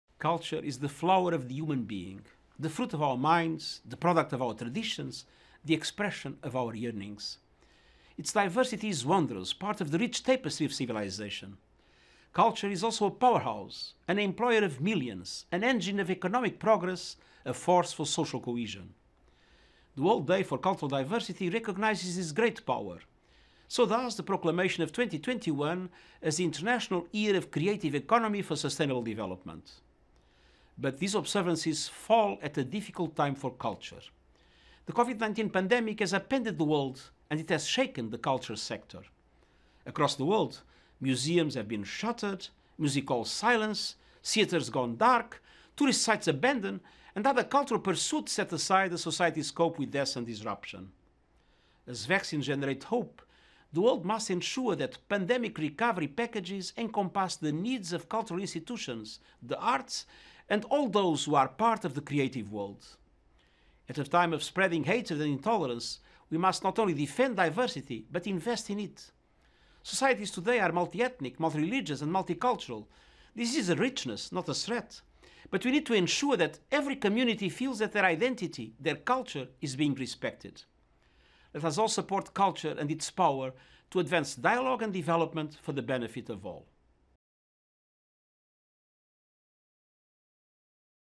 21 de maio, Dia Mundial da Diversidade Cultural para o Diálogo e o Desenvolvimento
Em 21 de maio as Nações Unidas marcam o Dia Mundial da Diversidade Cultural para o Diálogo e o Desenvolvimento.

Em sua mensagem para a data, o secretário-geral das Nações Unidas, António Guterres, lembrou que, em um momento de disseminação do ódio e da intolerância, não devemos apenas defender a diversidade, mas investir nela. “As sociedades hoje são multiétnicas, multirreligiosas e multiculturais. Isso é uma riqueza, não uma ameaça”, disse Guterres.